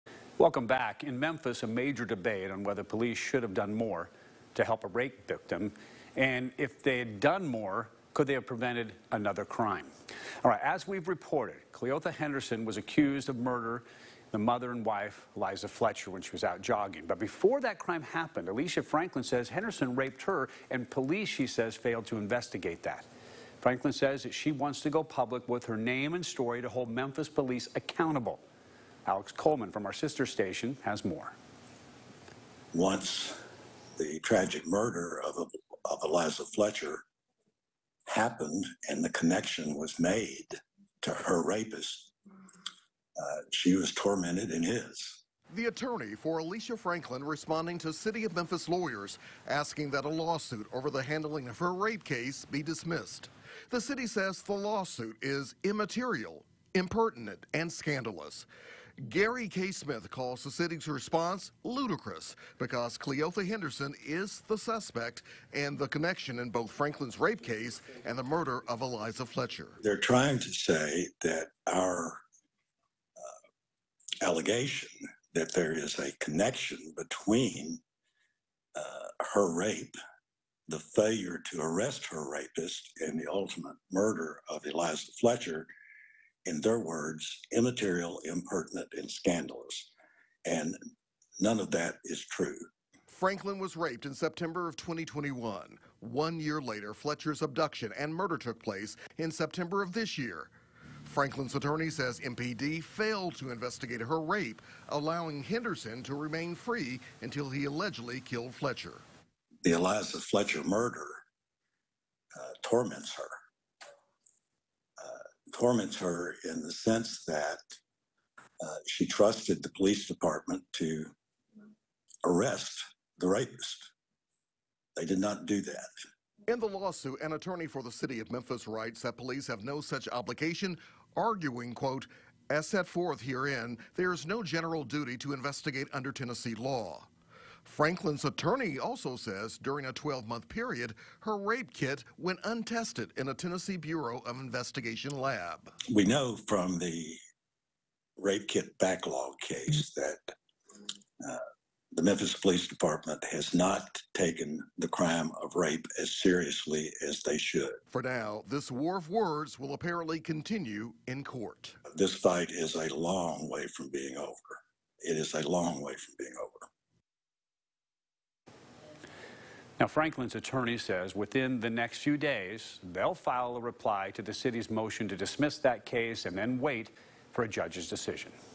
Lawyer says city's motion to dismiss Alicia Franklin lawsuit is 'ludicrous'
Lawyers for the City of Memphis want a judge to throw out a lawsuit filed by a woman who says police failed to properly investigate her rape, arguing that police have no duty under law to investigate.